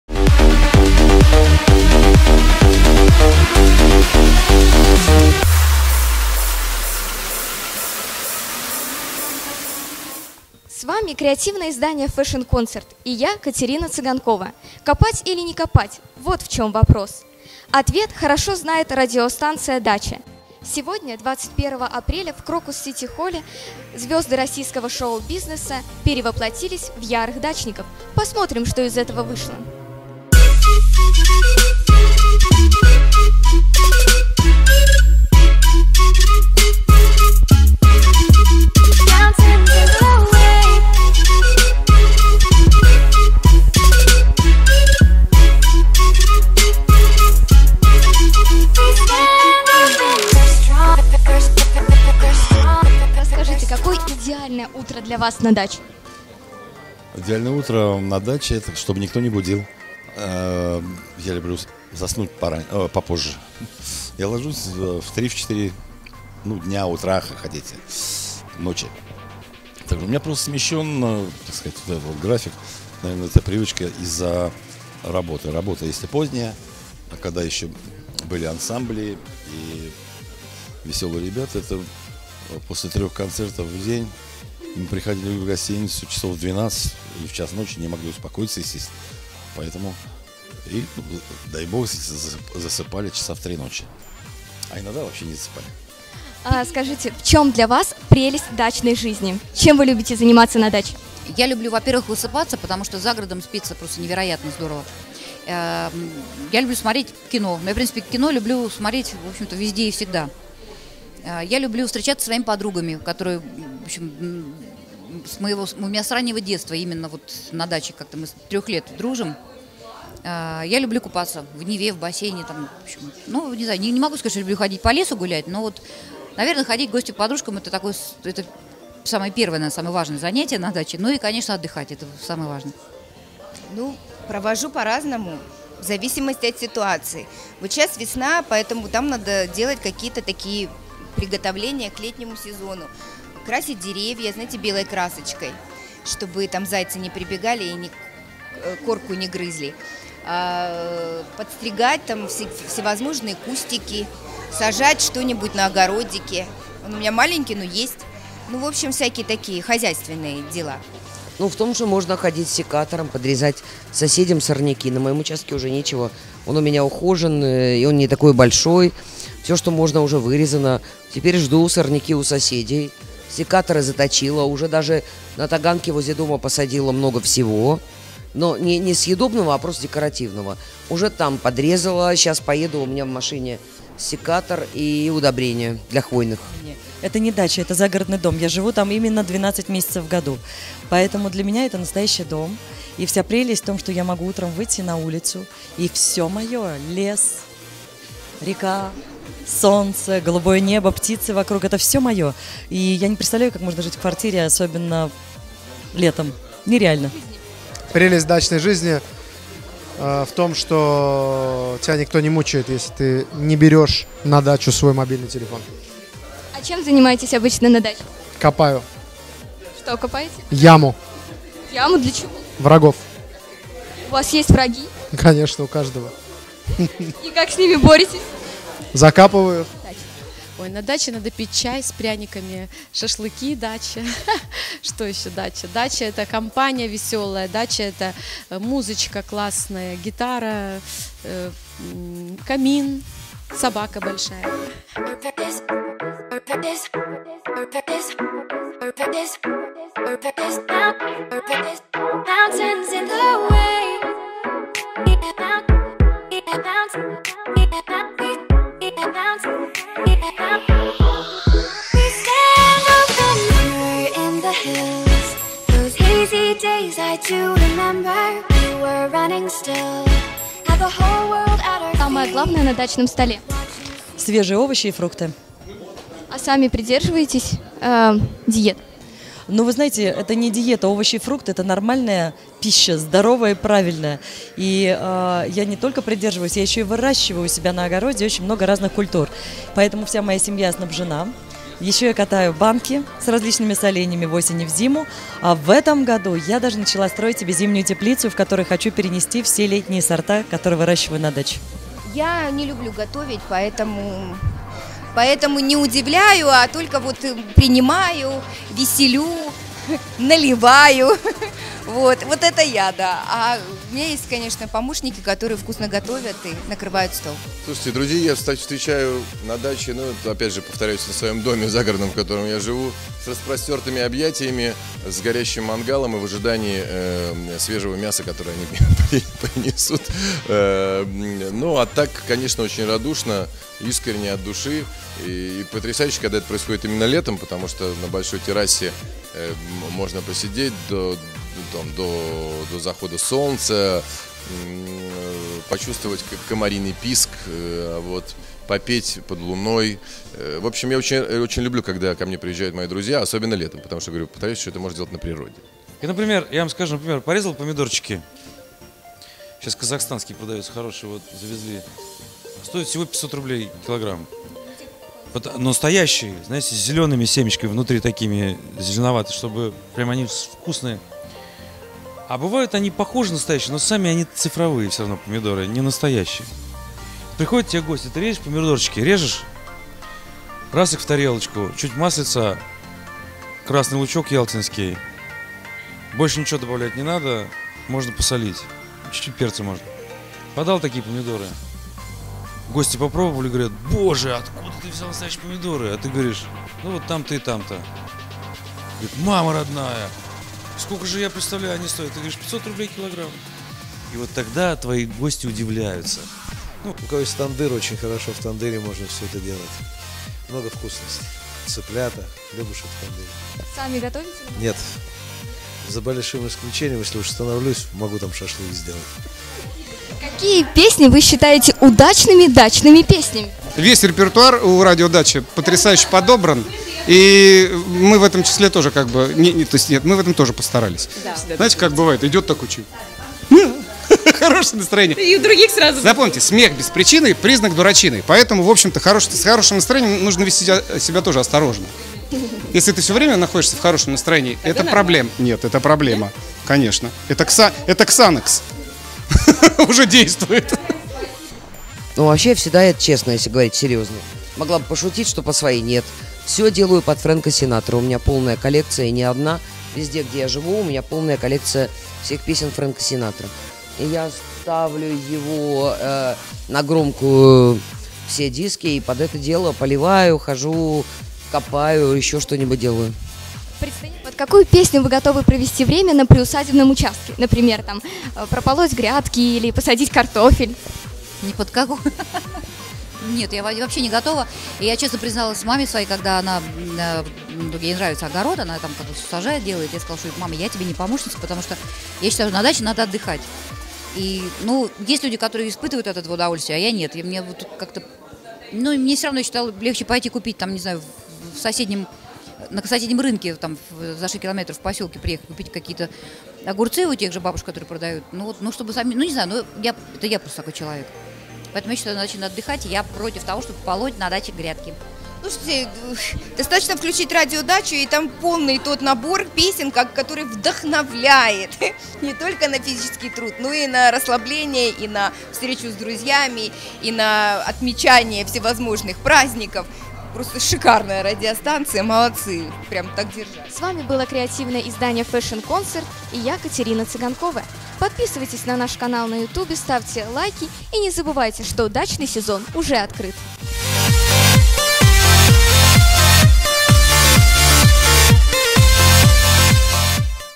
Удачные ноты на дачной музыкальной волне. «DISCO Дача» 2016
Настоящий артист — народный. Настроить одну частоту с любителями загородных утех и открыть новый дачный сезон на сцене Crocus City Hall 21 апреля 2016 не составило труда организаторам  концерта «DISCO Дача».